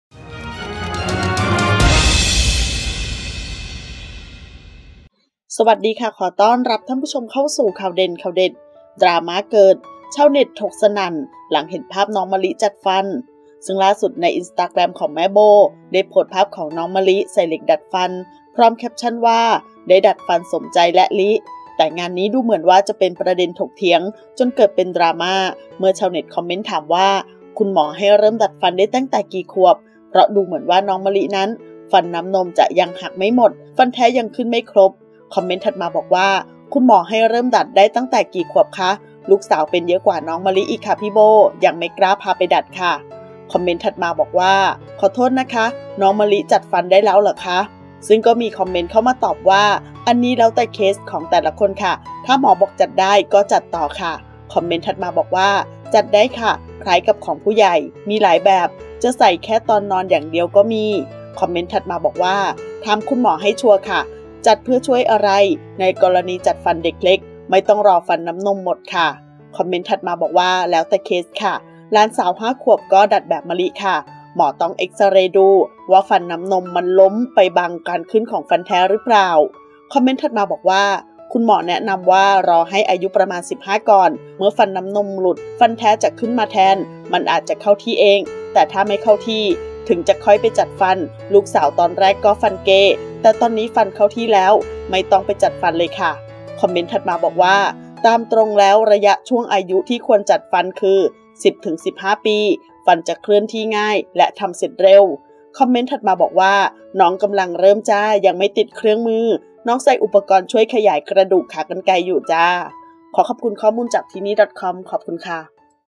ดราม่าเกิด! ชาวเน็ตถกสนั่น หลังเห็นภาพน้องมะลิจัดฟัน
ขอบคุณที่มา :
ขอขอบคุณข้อมูลจาก: